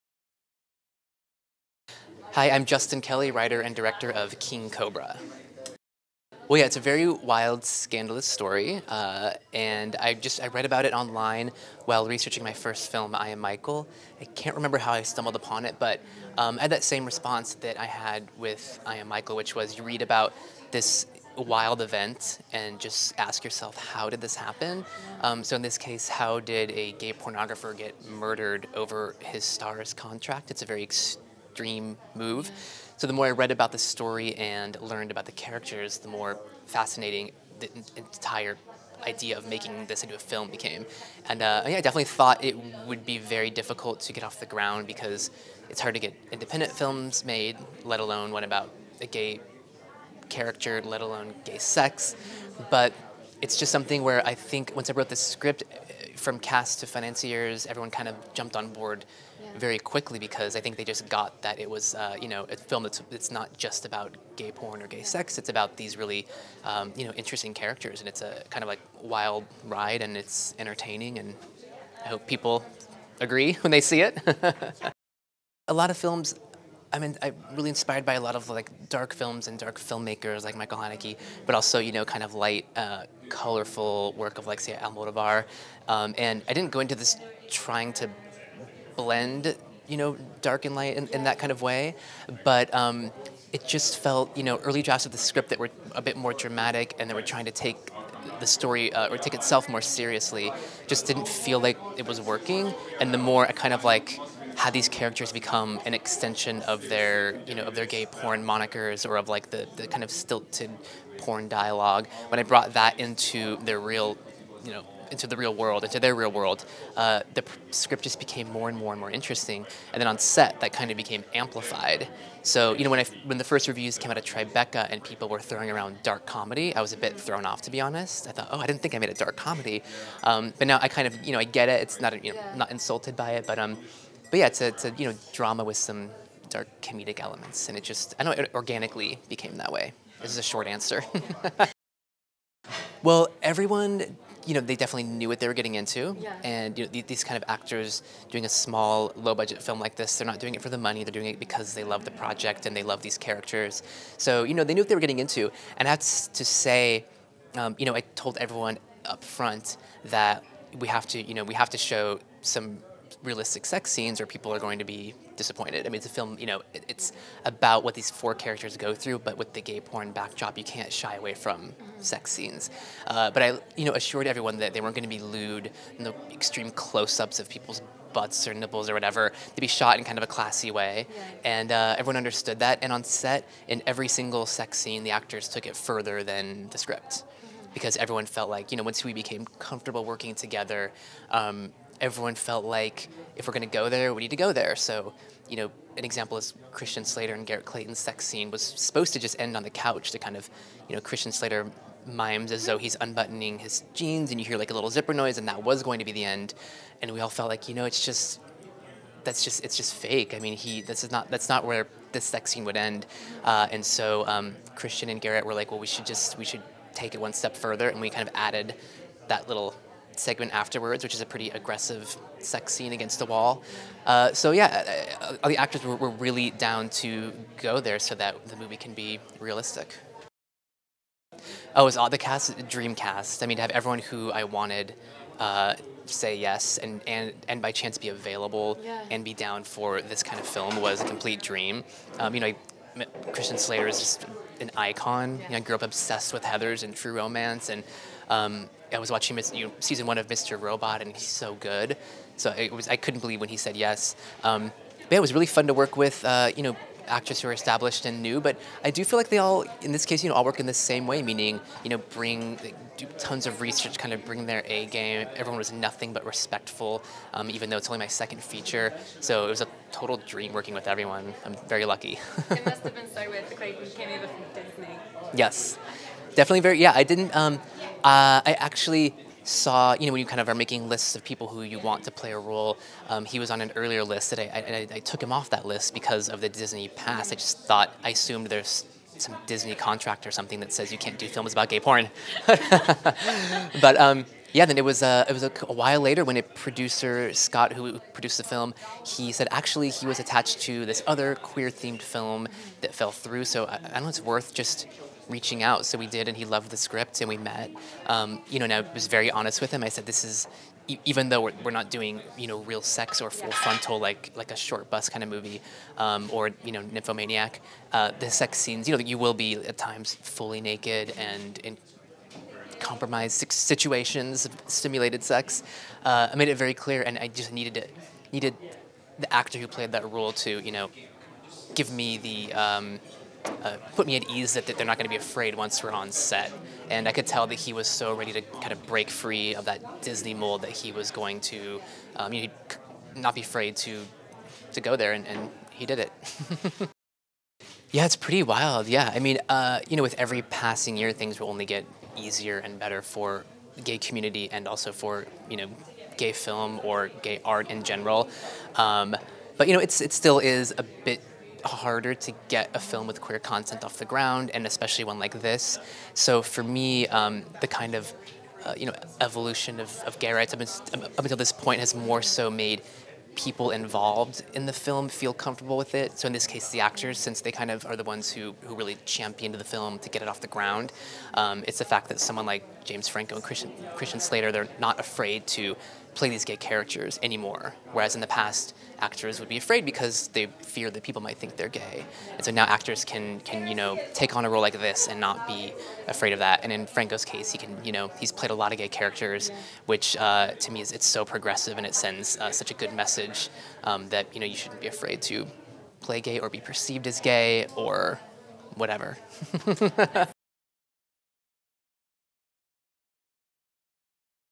King Cobra Director Justin Kelly Interview
Red Carpet News TV talks to King Cobra Director Justin Kelly at the BFI London Film Festival 2016.